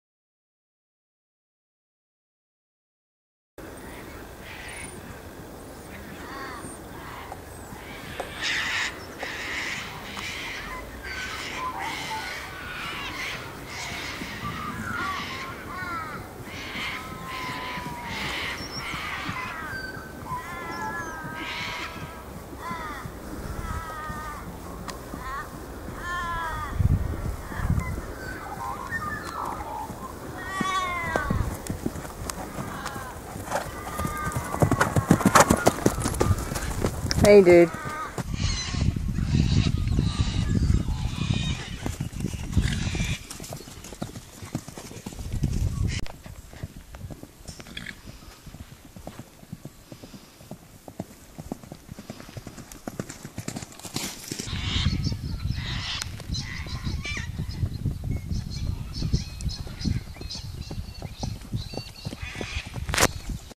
Skipper & Cy races to their yards- Appi vs the Anglo Arabian
An the good old Appi always loses.